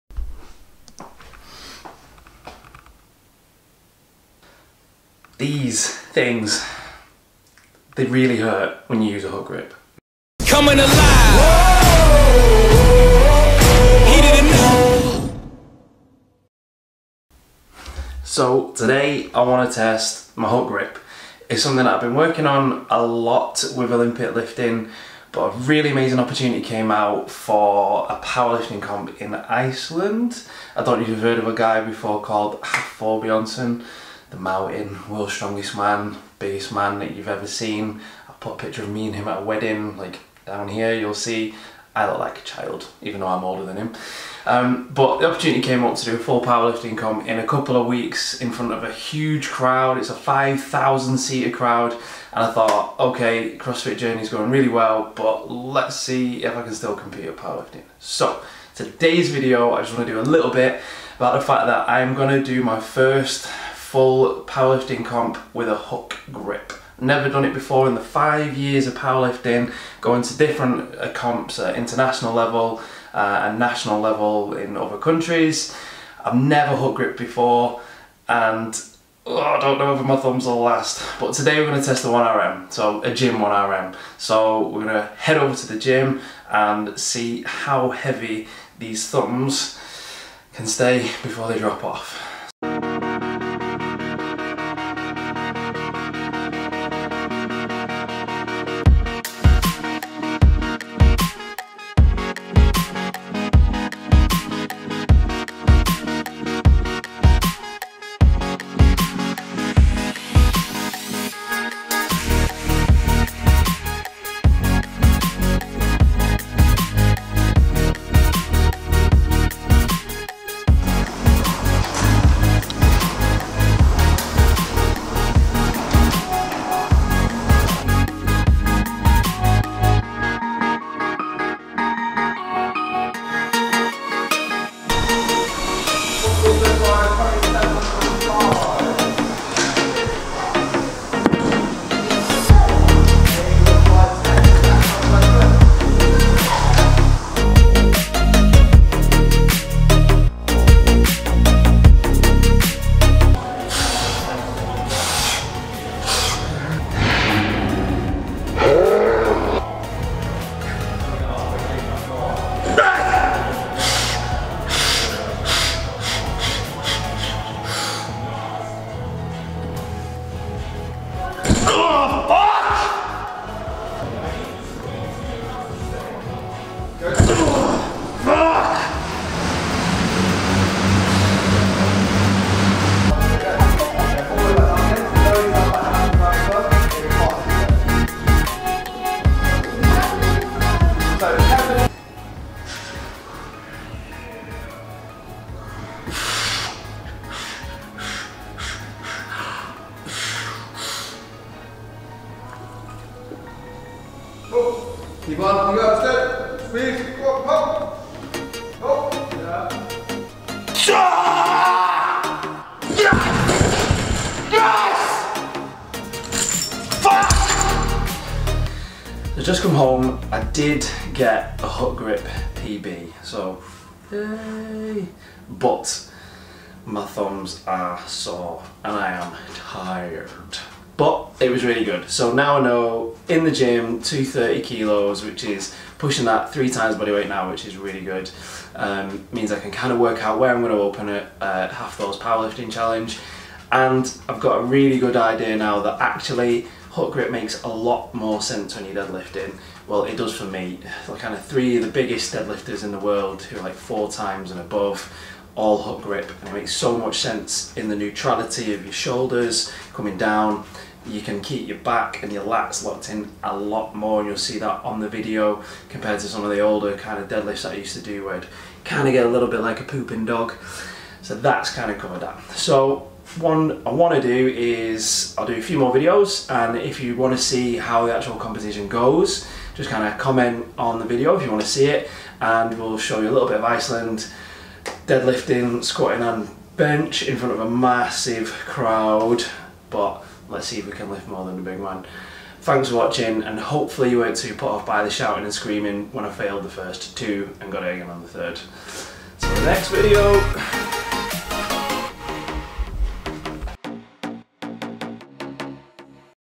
DEADLIFTING MY THUMBS OFF (hook grip test)
DEADLIFT HOOKGRIP TEST did I hit that 3 x Bodyweight lift in the gym?

I recently got the chance to sign up to Hafthors Powerlifting Challenge taking place in Iceland on the 15th December.

As I continue my journey into competitive Crossfit this was a powerlifting opportunity I couldn't turn down so decided to throw in some heavy training and use a hook grip at the competition. Today was my gym test to base my numbers running up to the event.

Follow me over at

Facebook Scott Brits